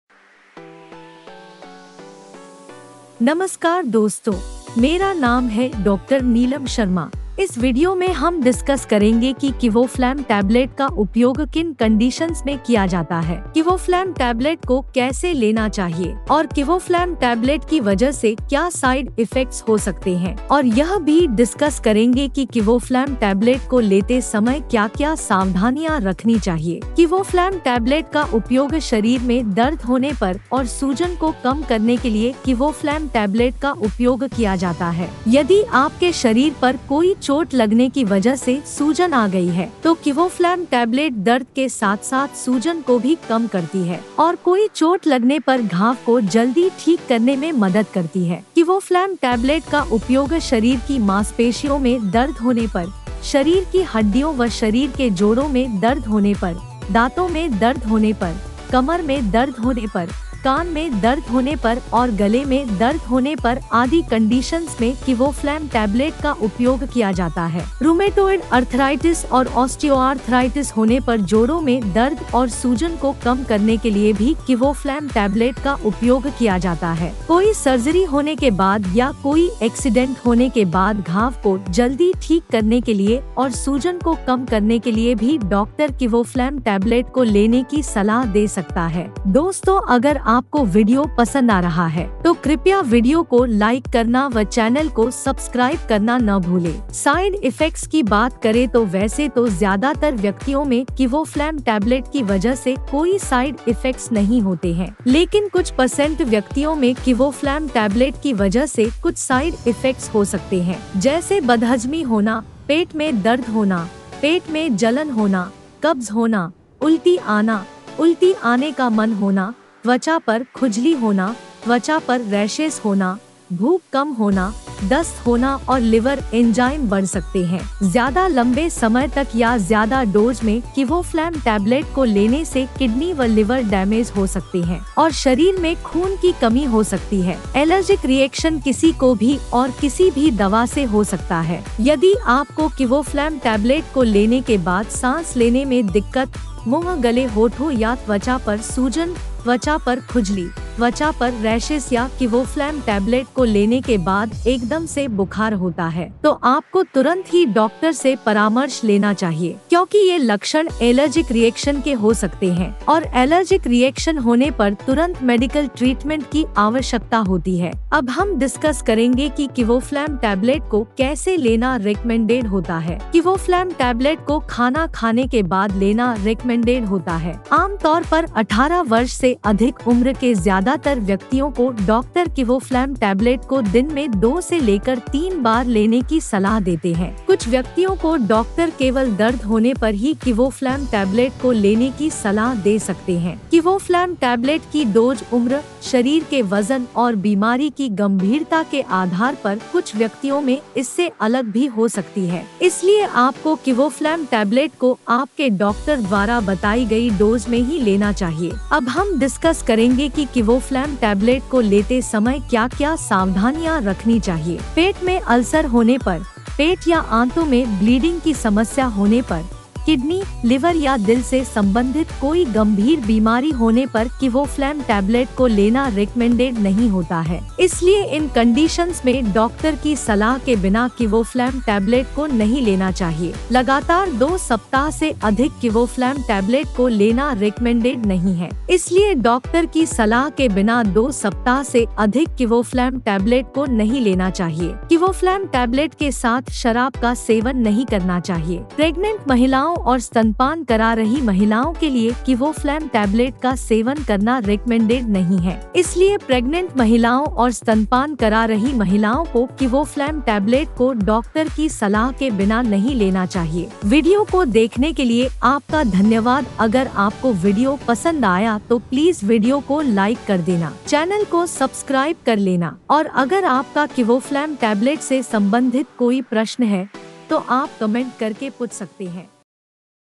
Kivoflam Tablet
kivoflam tablet uses & side effects in hindi | kivoflam tablet ke fayde aur nuksan | how to use kivoflam tablet. in this video, we will discuss what are the uses and benefits of kivoflam tablet. How to use kivoflam tablet. We will also discuss what are the side effects of kivoflam tablet. Watch the complete video for detailed review of kivoflam tablet in hindi.